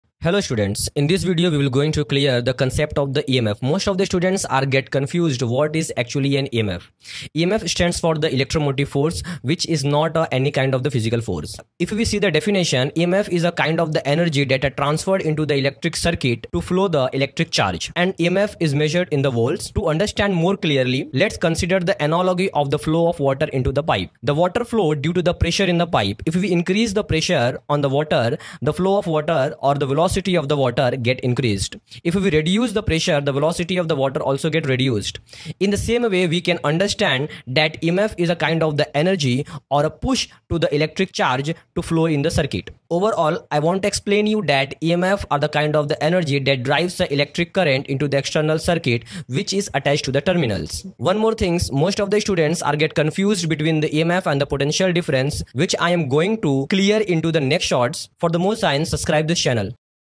What is EMF? | EMF - ELECTROMOTIVE FORCE | Electric Current
Electromotive force (EMF) is the energy per unit of charge that is provided by a source, such as a battery or a generator, to move electrons through an electrical circuit.

EMF can be thought of as a measure of the "push" or "drive" that causes electrons to move from the negative to the positive terminal of a battery or generator, creating a flow of electric current. EMF is not a force in the traditional sense, but rather a potential difference or voltage that exists between two points in an electrical circuit.

EMF is measured in volts (V) and represents the amount of energy that is required to move a unit of charge (such as an electron) through the circuit. The greater the EMF, the more energy is available to move electrons through the circuit, and the greater the potential difference between the two points in the circuit.

It's important to note that EMF is not a source of energy itself, but rather a measure of the potential energy that can be converted into kinetic energy (i.e. the energy of moving objects) as electrons flow through the circuit.